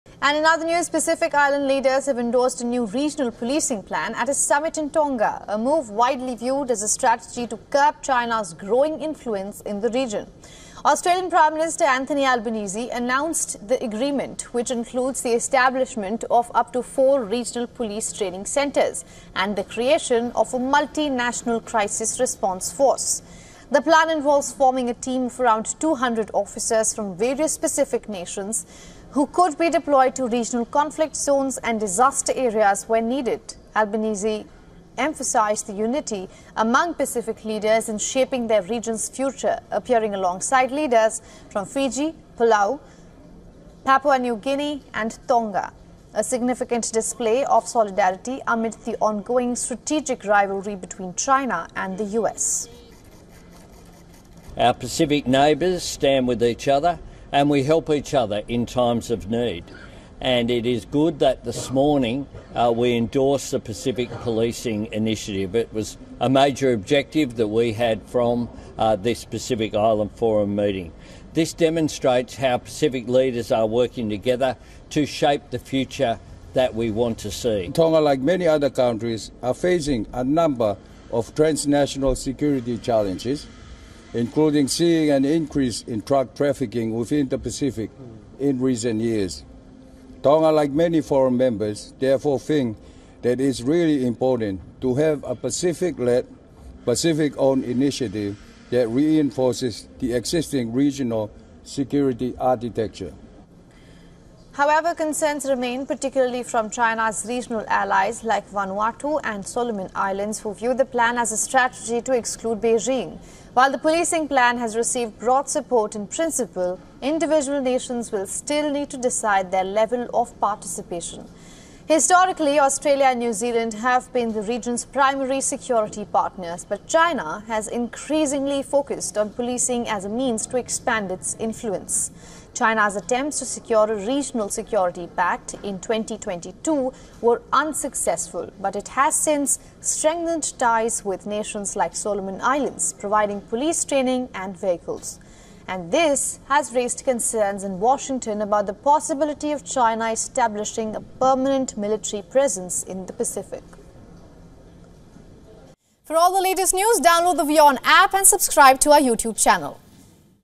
Pacific Island leaders back joint regional police force in Tonga meeting | WION
Pacific Island leaders endorsed a landmark regional policing plan Wednesday at a summit in Tonga, a contentious move seen as trying to limit China's security role in the region.